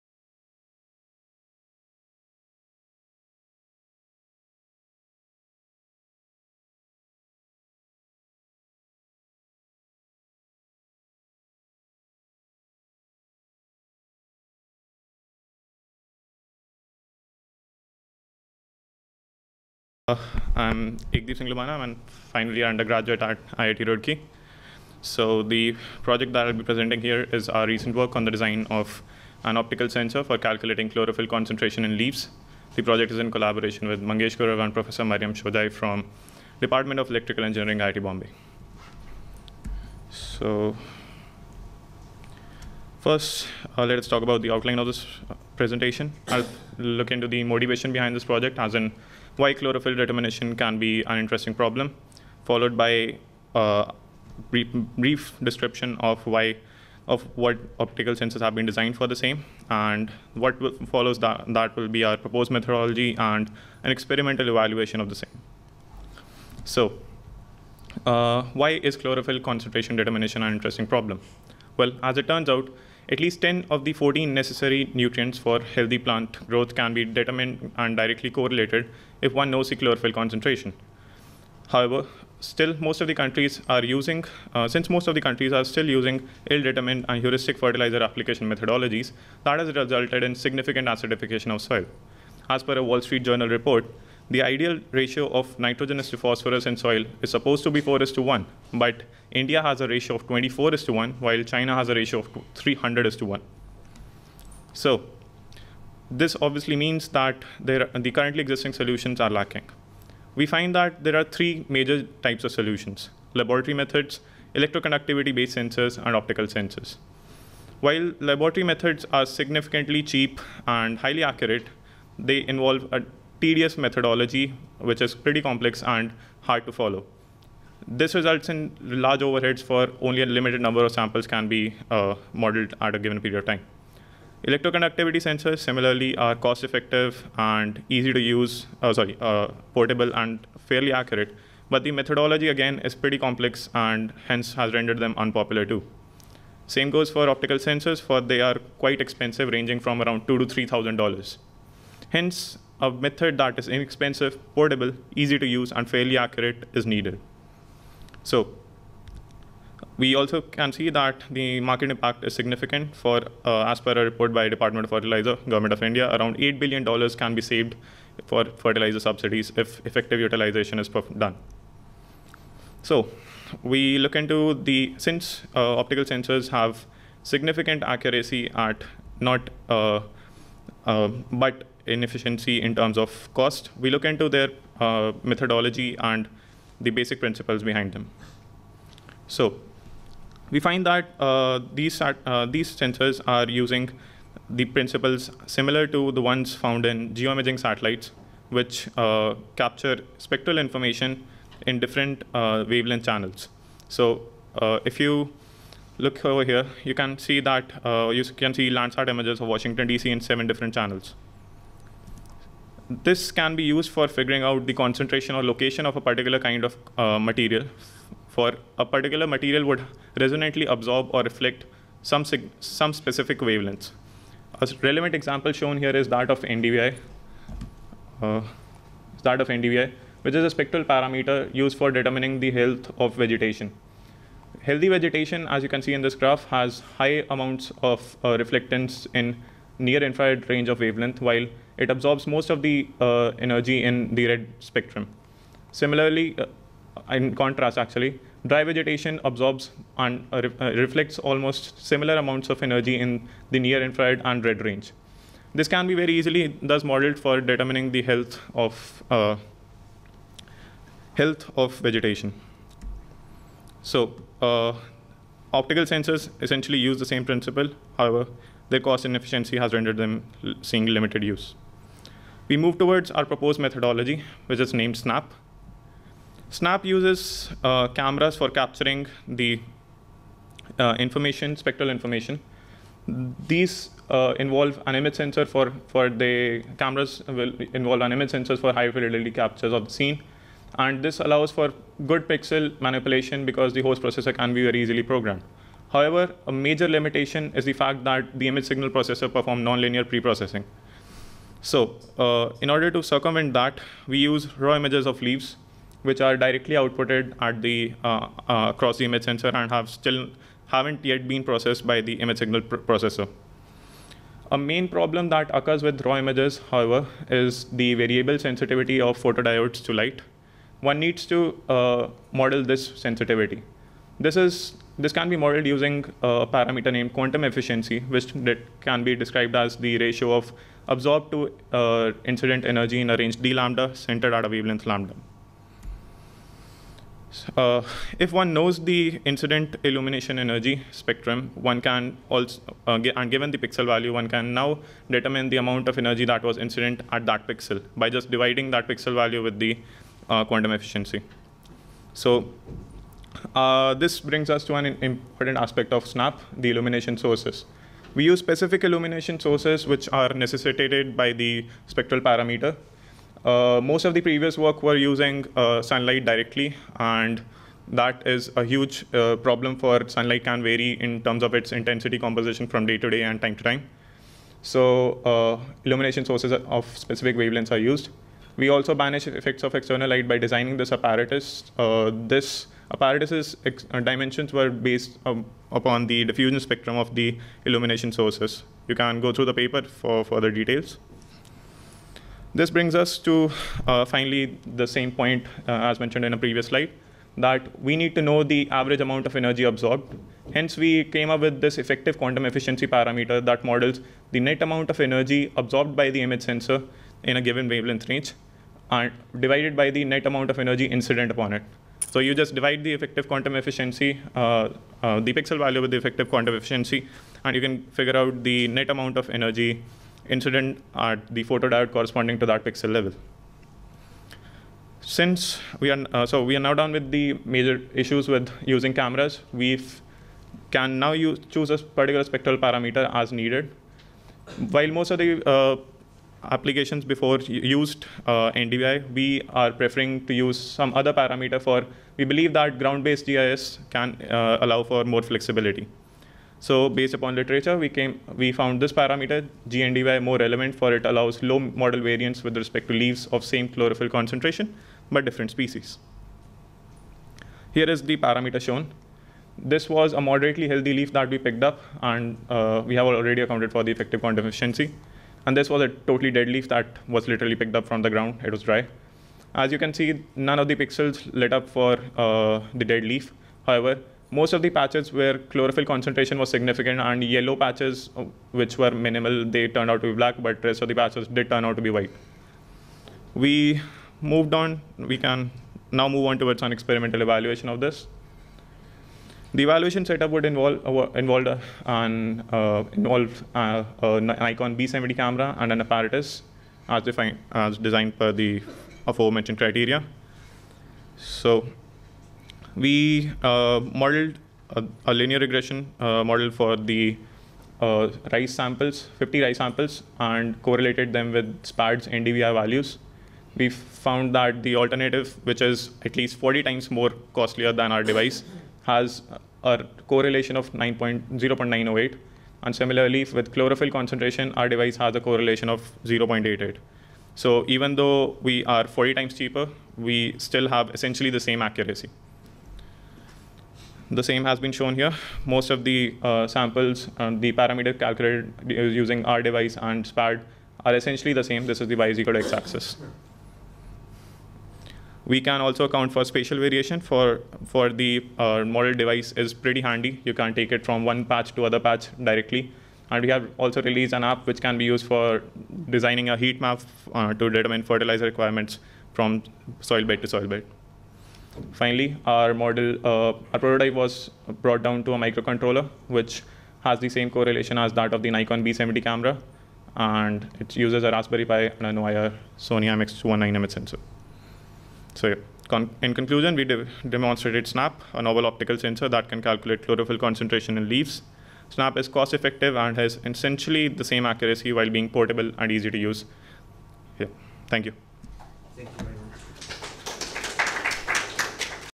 Snap: Chlorophyll Concentration Calculator Using Raw Images of Leaves
Authors:
Ekdeep Singh Lubana, Mangesh Gurav, Maryam Shojaei Baghini

Abstract:
Determination of nitrogen content, a major component of chlorophyll in plant leaves, is a demand. We propose SNAP, a handheld optical sensor device that uses RAW images, fixed apparatus design, and diffusion properties of illumination sources to determine leaf chlorophyll content. Remote sensing based spectral parameters, which are known for their correlation with the chlorophyll concentration, are used for quantitatively determining the chlorophyll concentration. While RAW images help generate a linear relationship between pixel values and corresponding incident illumination, a fixed apparatus design banishes external illumination's influence on chlorophyll estimation and brings geometrical consistency.